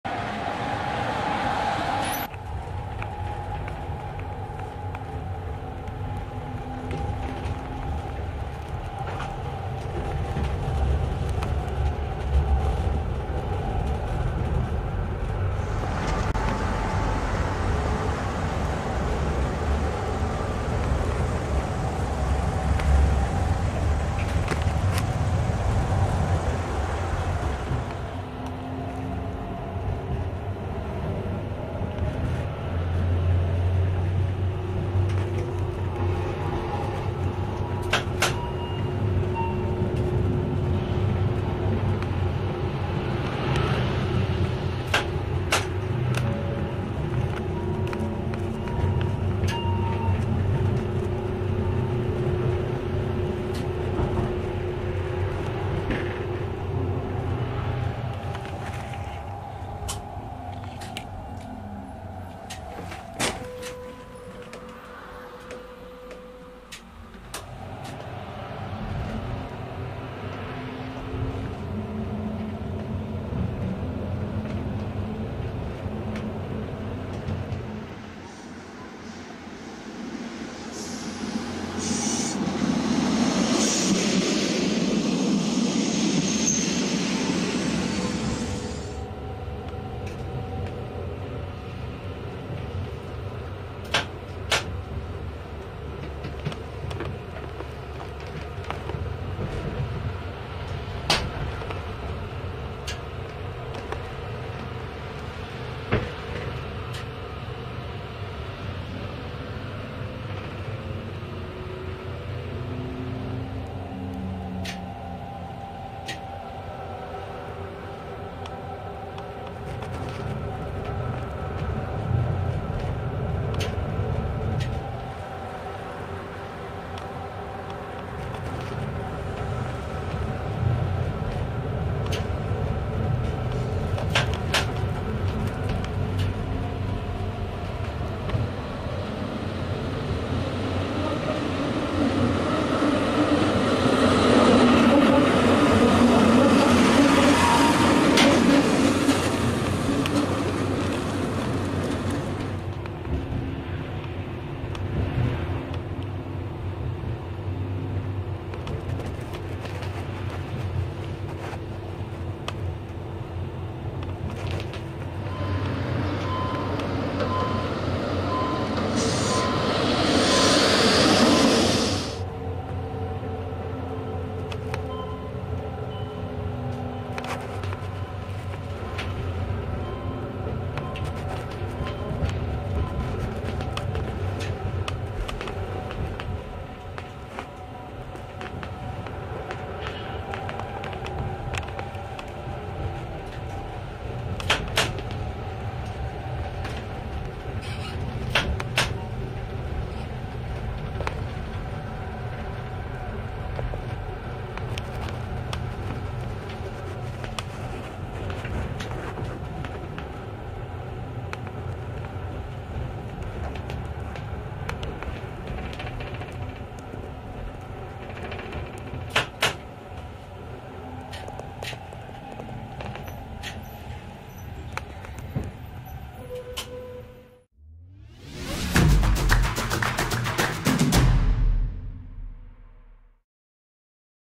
Zeitreise: Im Führerstand von Suhr nach Aarau (2010/2021)
Im November 2010 wurde die Neubaustrecke der Wynental- und Suhrentalbahn zwischen Aarau und Suhr eröffnet. Damit verschwanden die Züge der WSB auf diesem Abschnitt von der Strasse. Einsteigen zu einer kleinen Zeitreise: Im Video vergleichen wir die Fahrt von Suhr nach Aarau aus der Führerstands-Perspektive.

Herzlichen Dank an Urs Bader für das Video aus dem Jahr 2010.